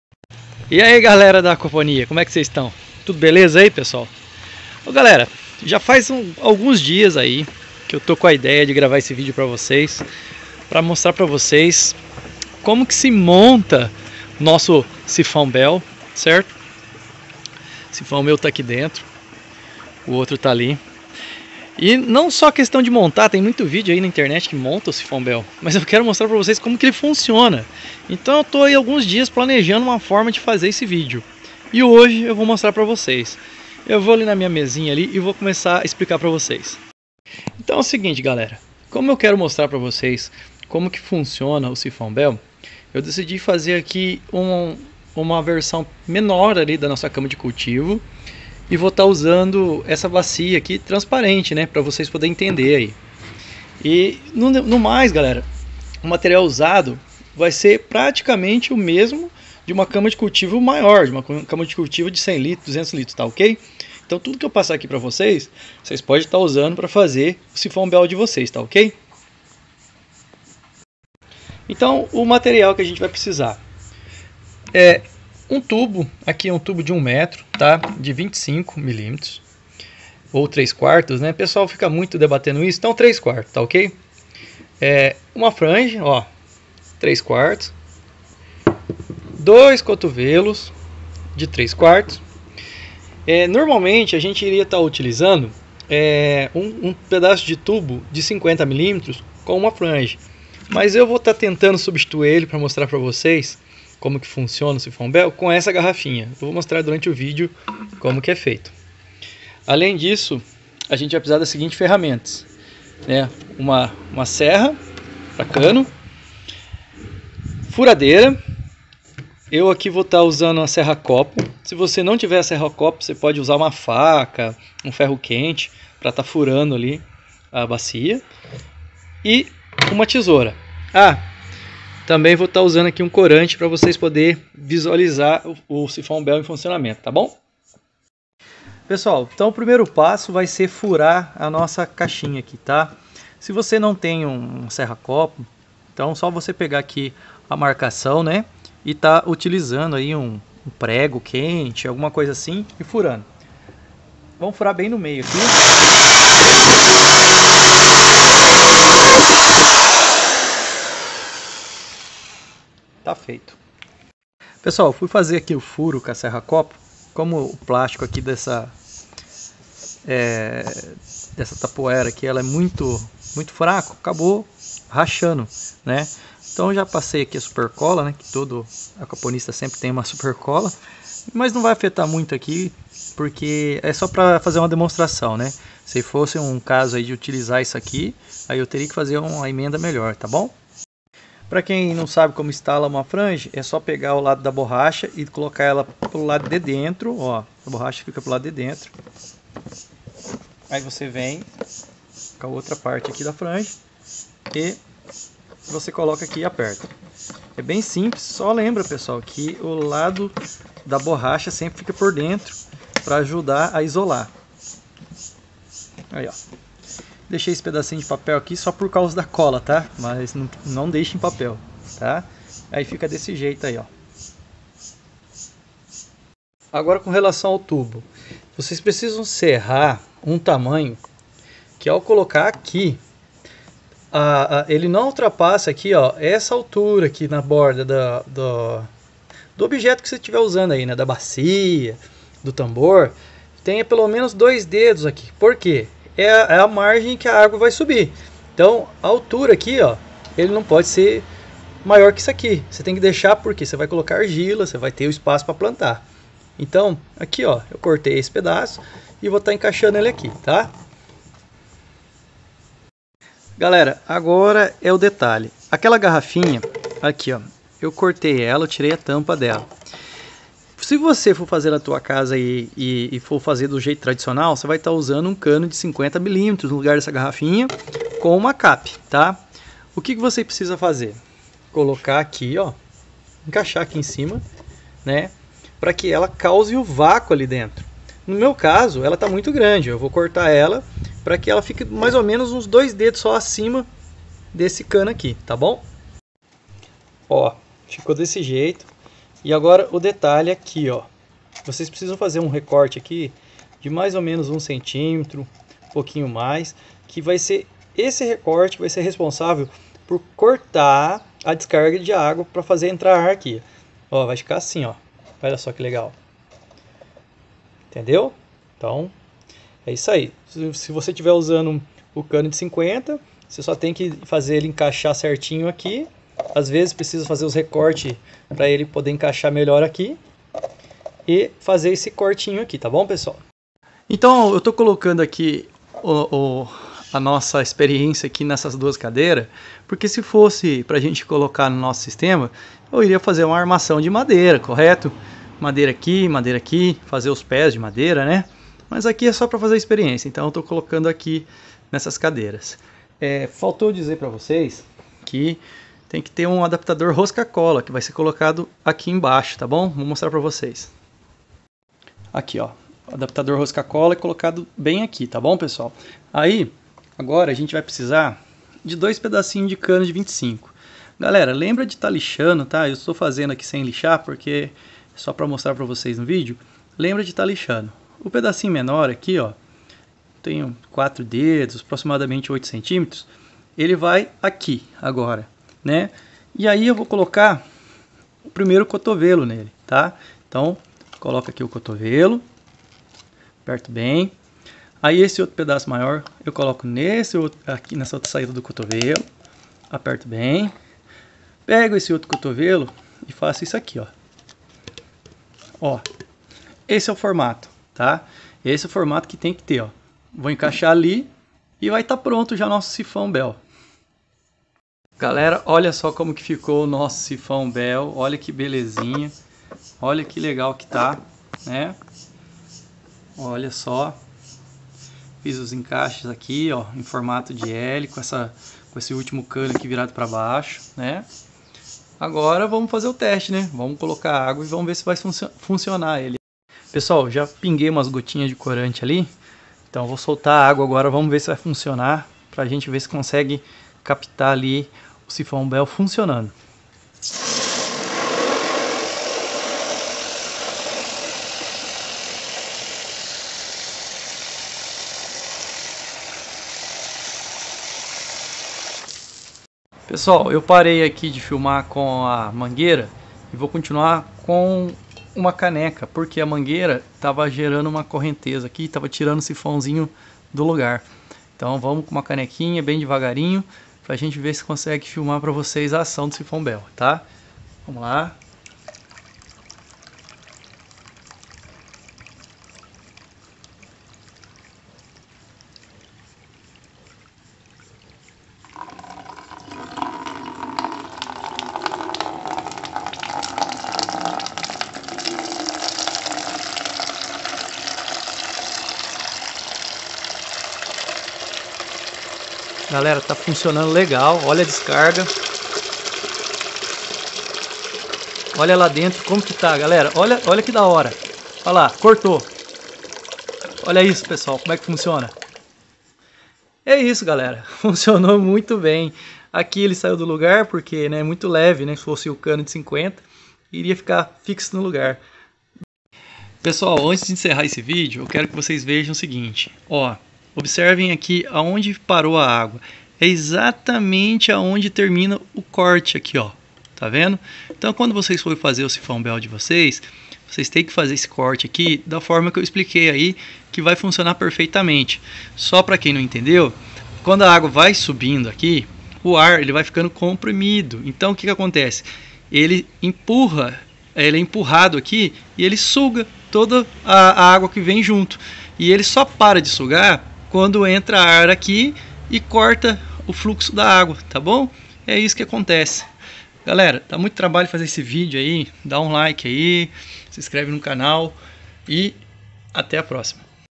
Como fazer um sifão Bell e entenda como ele funciona por dentro, assistam é sensacional !
Neste vídeo além de mostrar como montar um sifão Bell. Também mostro como ele funciona por dentro.